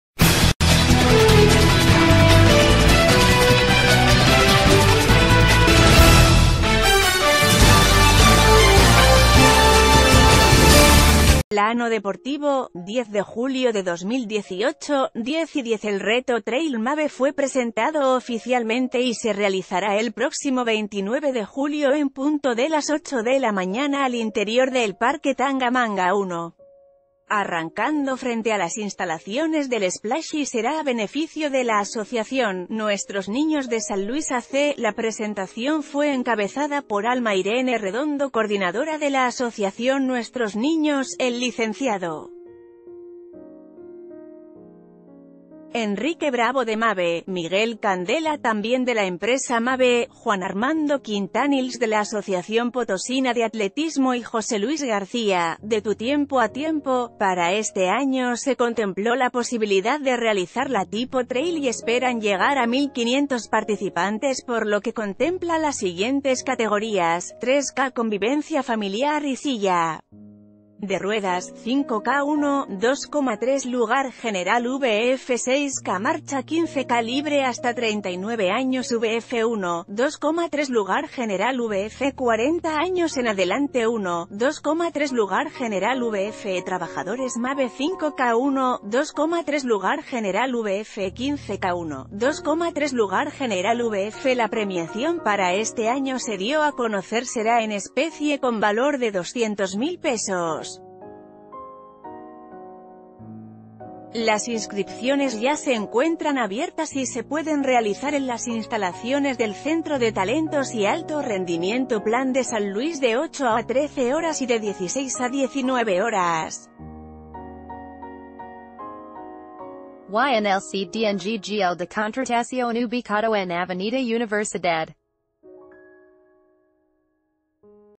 Presentaron el Reto Trail Mabe 2018
Plano Deportivo | 10 Julio 2018 | 10:10 El Reto Trail Mabe fue presentado oficialmente y se realizará el próximo 29 de julio en punto de las 8 de la mañana al interior del Parque Tangamanga 1 arrancando frente a las instalaciones del Splash y será a beneficio de la Asociación "Nuestros Niños de San ...